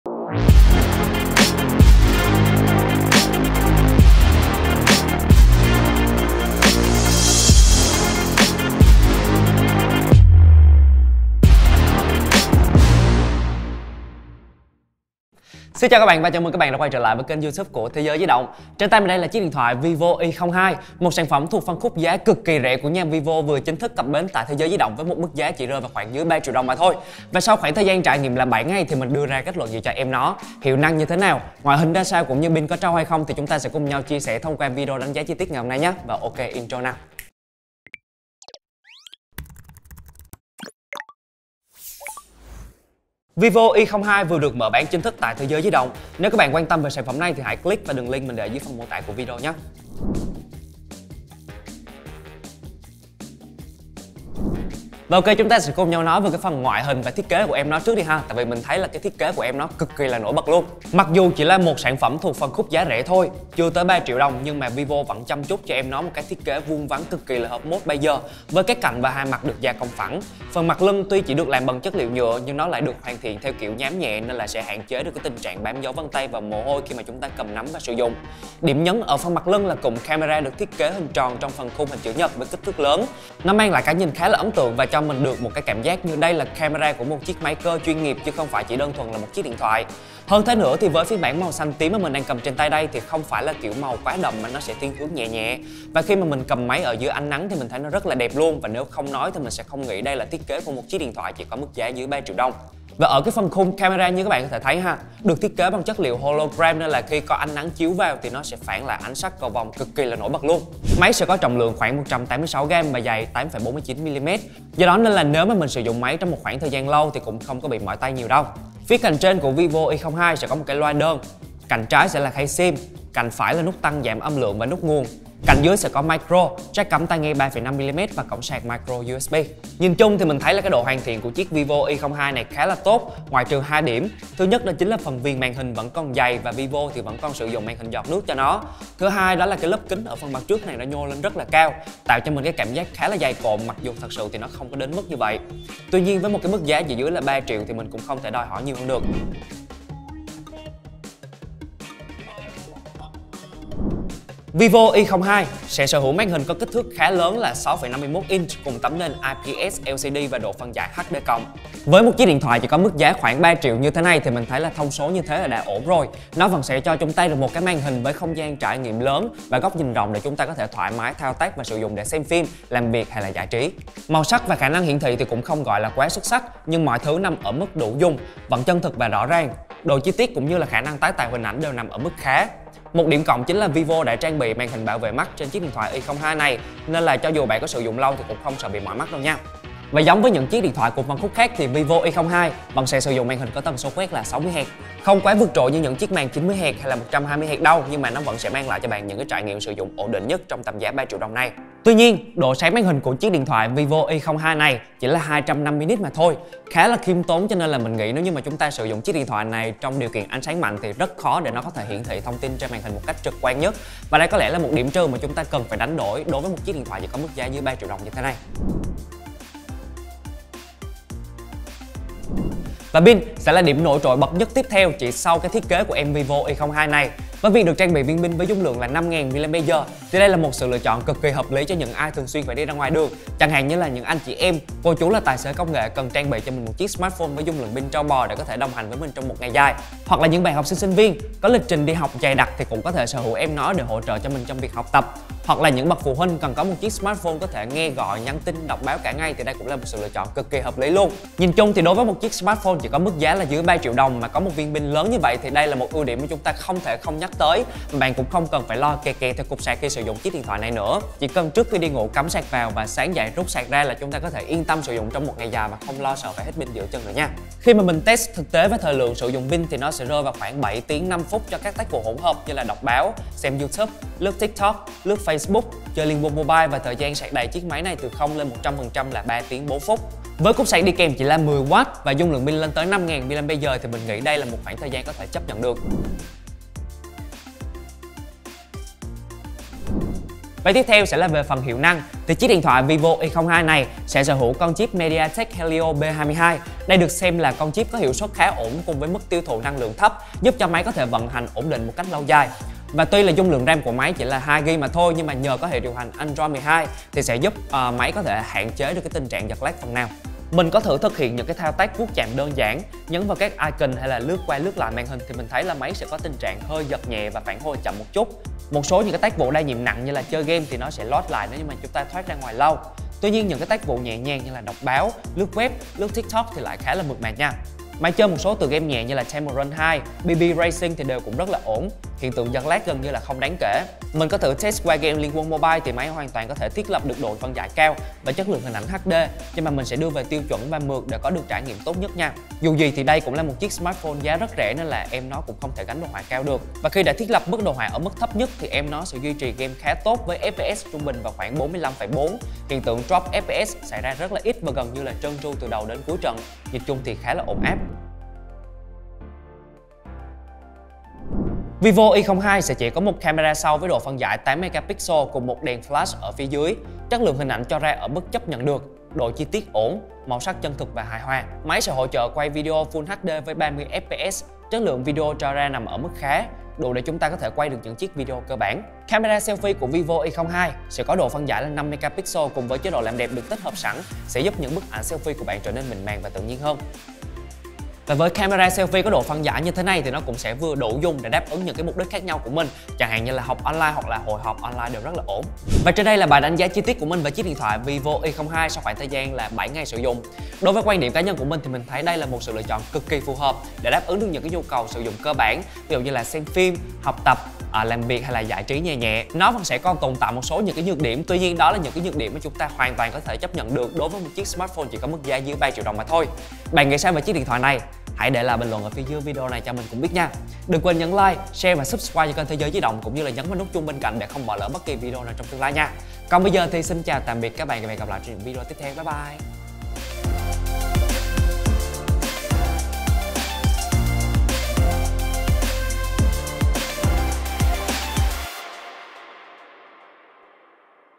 Đánh giá chi tiết vivo Y02 - Trong phân khúc 3 TRIỆU liệu có xứng đáng? | Thế Giới Di Động
Cuối cùng thì vivo Y02 cũng đã cập bến tại Thế Giới Di Động! Với mức giá vô cùng hạt dẻ, chỉ dưới 3 triệu đồng thì em nó sẽ có những tính năng gì nổi bật? Cùng xem nhé!

00:00 Intro
00:51 Thiết kế
02:29 Màn hình
03:39 Pin
04:46 Hiệu năng
05:58 Camera
06:35 Outro